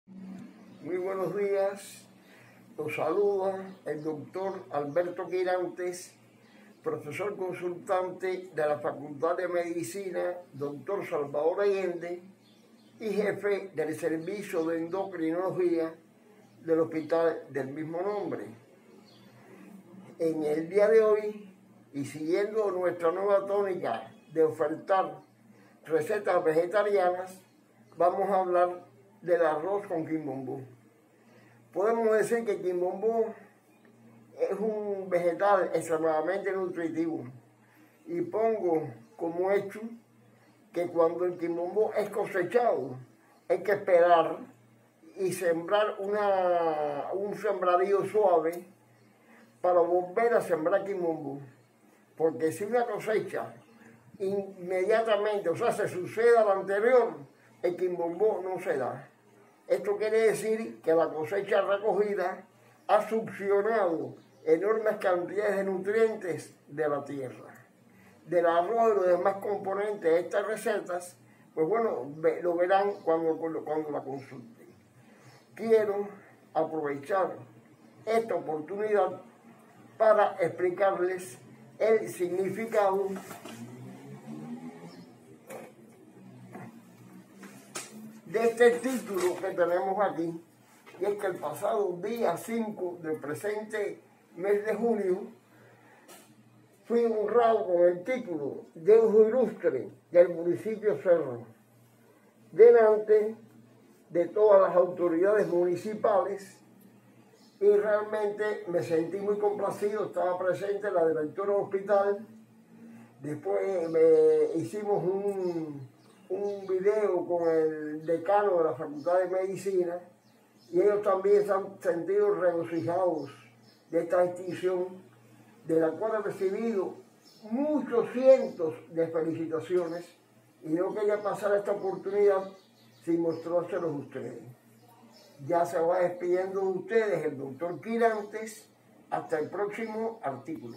CocinaDeCuba | Receta vegetariana de arroz con quimbombó a la cubana
La ventaja del arroz con quimbombo es que es una receta muy diversa y lo mismo se puede preparar con pocos ingredientes que añadirle otros si se desea.